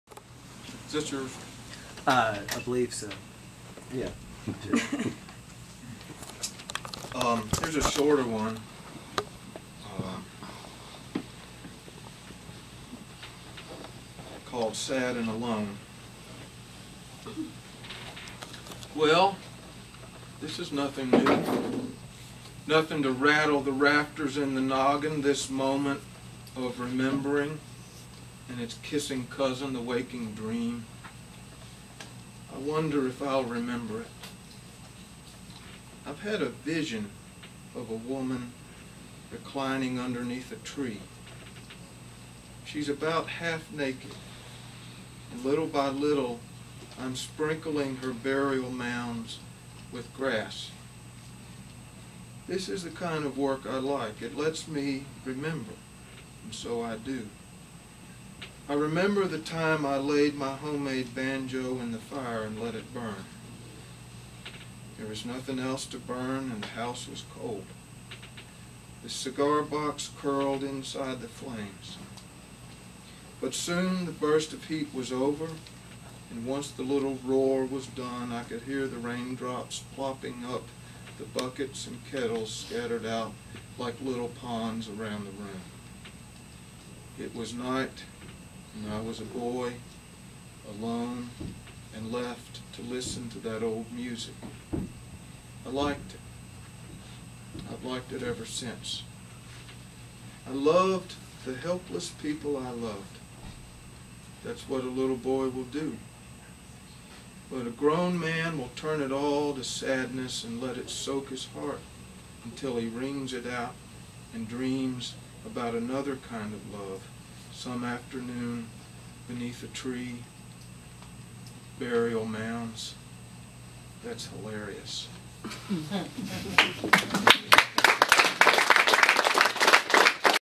Maurice Manning - Sad and Alone - April 2010.avi
Poet Maurice Manning reads his poem SAD AND ALONE, from his collection THE COMMON MAN.  This reading occurred at The Morris Book Shop in Lexington, Kentucky, April 2010.